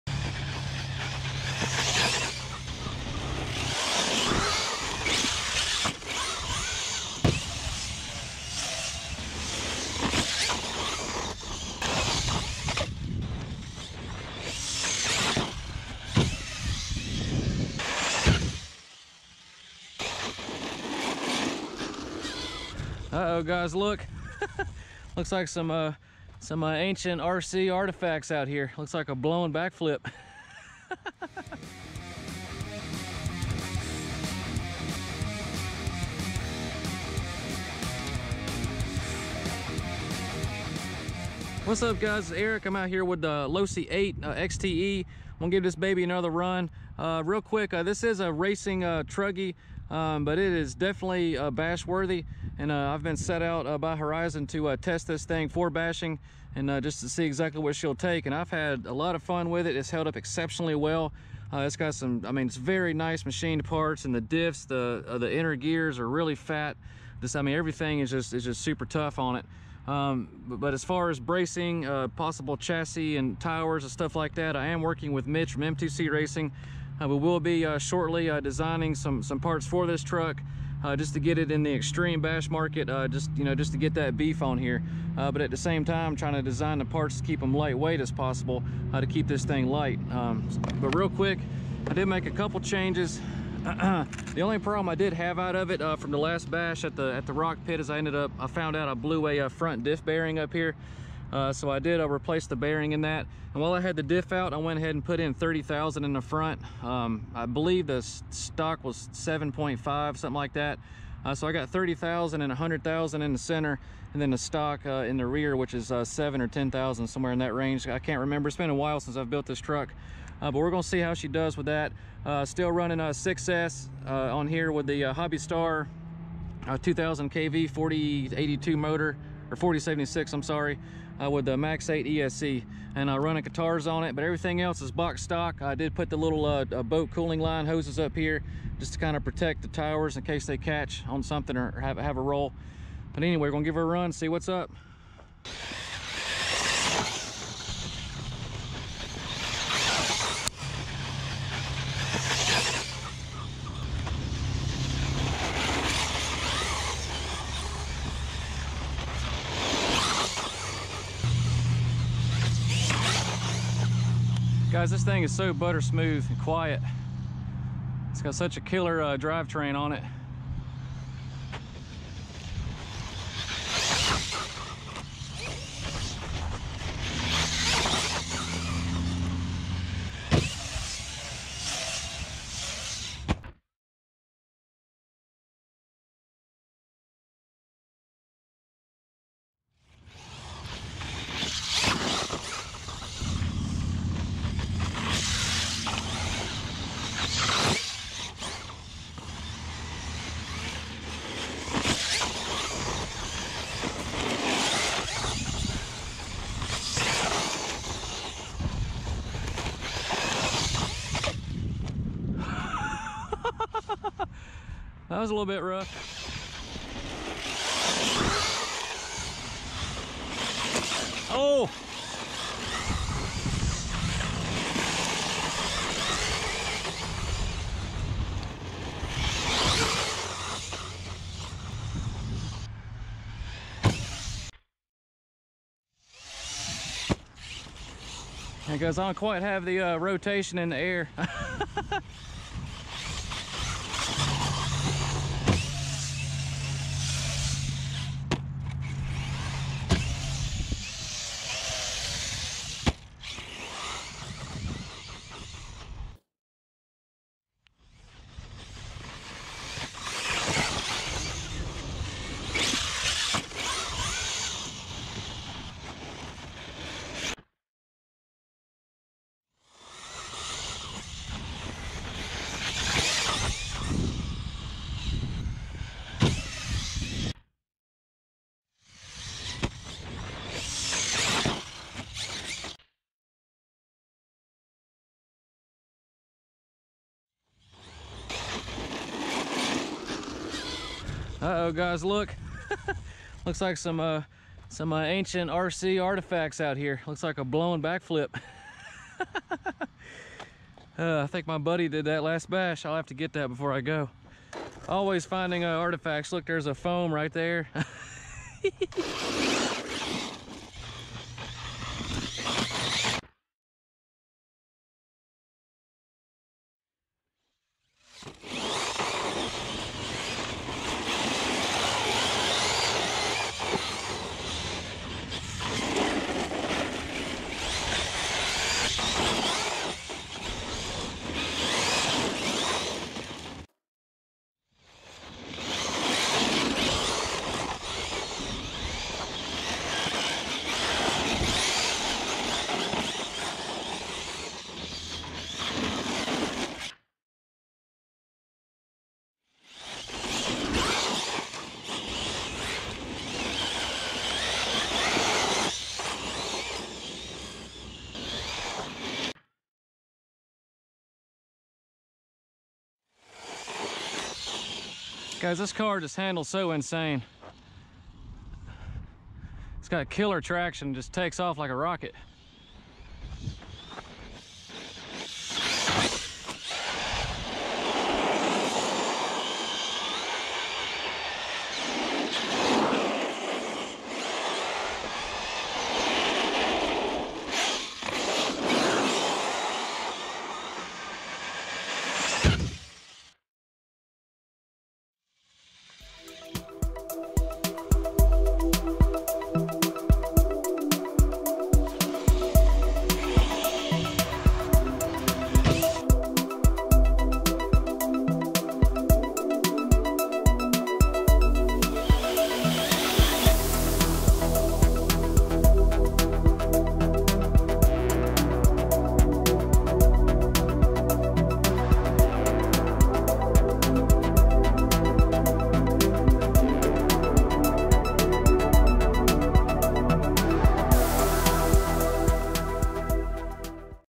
LOSI 8ight XT/e 6s! "BASHING LAND ROCKET!!"😱High speed rips/drifts and air!
Out with the Losi 8ight XT/e electric Truggy! For a racing spec rig, I'm totally blown away by it's overall durability and butter smooth handling and driving! Had a total blast just ripping her around and she fly's so balanced! Hope to be working with M2C Racing soon for some future beef for this rig to get her in the extreme bash market! So far the only issues I've had are a blown front diff bearing and a cracked rear bulkhead which I will go over in my next vid with it. Overall I'm still SUPER impressed with what this thing takes almost box stock! Building it was a pleasure and the parts are such well engineered and machined parts! HUGE shoutout to Horizon and TLR for the opportunity once again!